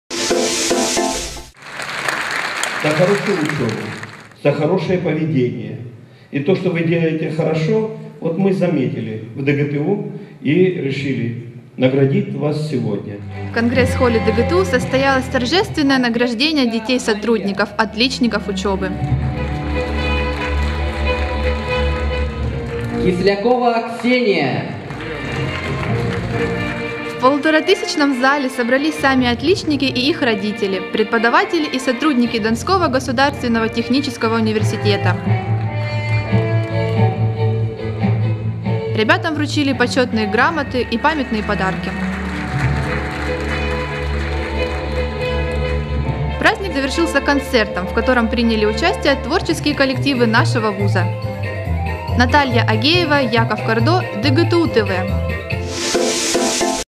Награждение отличников
ДГТУ_ТВ, Выпуск "ДГТУ_News" от 5 декабря 2013г., «Награждение отличников»
ВЫБИРАЙТЕ КАЧЕСТВО 720p HD!

Состоялось традиционное чествование детей сотрудников университета, имеющих отличные успехи в учёбе. В конгресс-холле ректор ДГТУ Б. Ч. Месхи в торжественной обстановке вручил им грамоты и денежные премии.